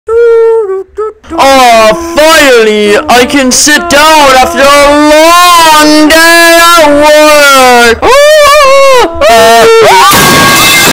Freddy sits down and dies (made by @Orangeballisthebestdodgeball )
After a long year of continuous work hours, Freddy finally get's to go home and see his only family, Chair. Sadly, he grows and breaks his chair, causing him to fall and break all the bones in his body, which in turn caused him to perish. And no one came to his funeral... or cared. My friend made this.